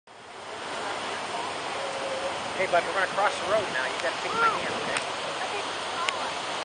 The Mighty Black River in Dexter
The Black River overflowing and flooding Dexter Municipal Building and a park